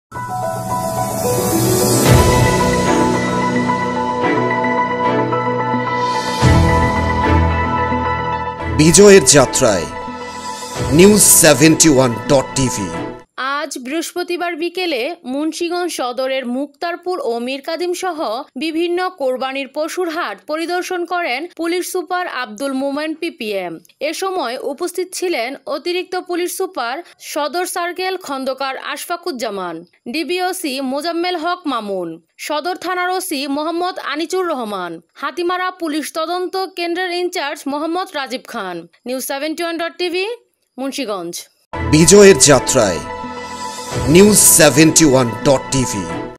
মুক্তারপুর ও মিরকাদিম সহ বিভিন্ন কোরবানীর পশুর হাট পরিদর্শন করেন পুলিশ সুপার আব্দুল মোমেন পিপিএম।
মুন্সীগঞ্জ সদরের মুক্তারপুর ও মিরকাদিম সহ বিভিন্ন কোরবানীর পশুর হাট পরিদর্শন করেন পুলিশ সুপার আব্দুল মোমেন পিপিএম।